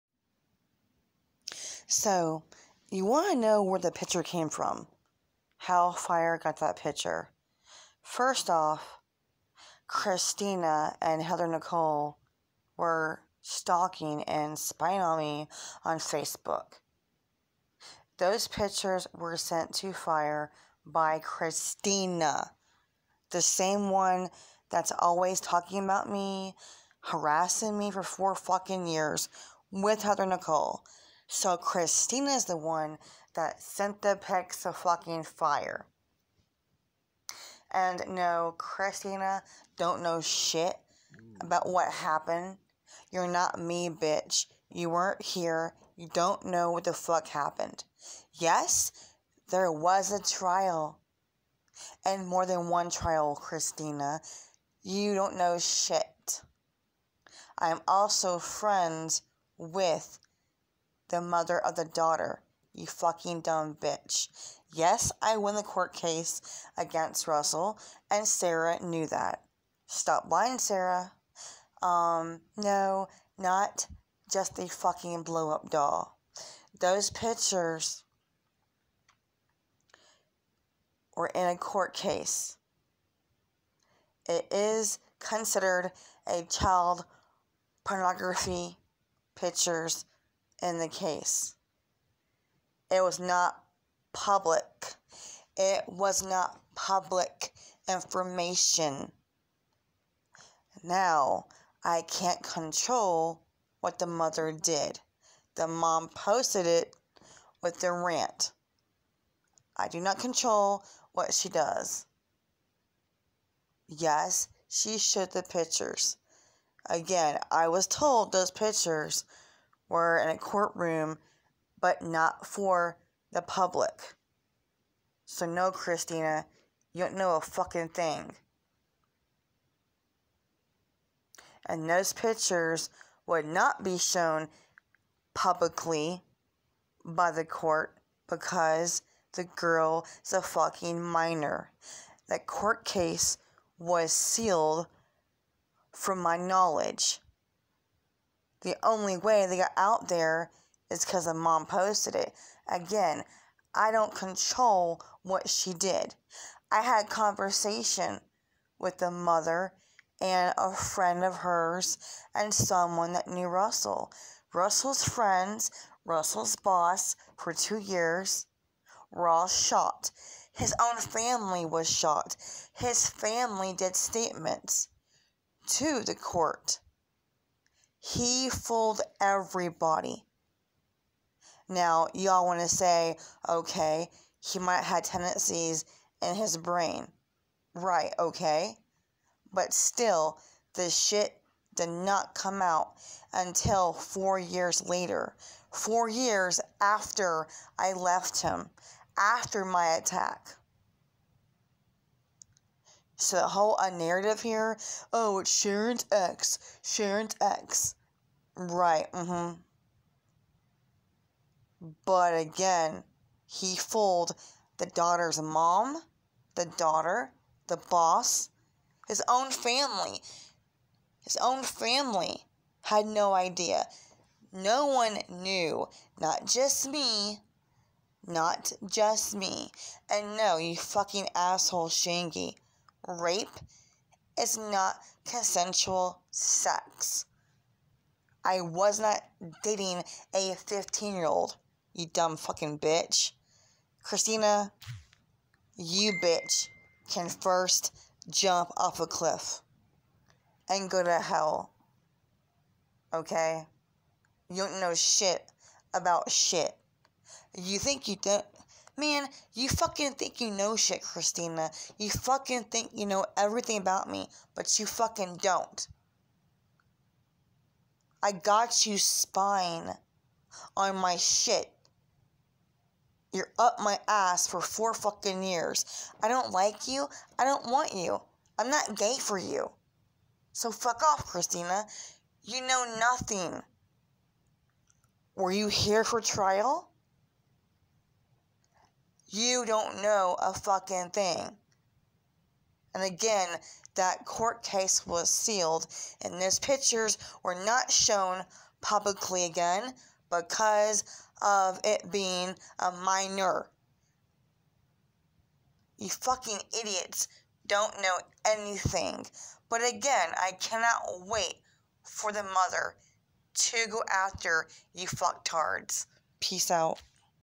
Christina DOESN'T know it all
Christina jumps on panel to try to give a background explanation instead of telling the truth and HER involvement. Christina and HN were stalking me me on FB she sent fire pics of a conversation I had with the daughter's mom bc WE ARE FRIENDS! These pics were not shown publicly bc of the court they were shown on fb bc the mom posted it to show for awareness,I don't control what that mom does...YES there was a trial you stupid hoe that's how much you DON'T KNOW b.c you don't do the research you just assume. THESE PICS WERE 100  %  CLASSIFIED AS CHILD PORN YOU IDIOT . I am NOT the only person that was fooled by Russell,his boss of 2 years, his friends and family too! I have document of my attack and the 3rd dental xray that was not shown on my channel so f u Christina you don't know shit u ugly beotch...keep reaching chic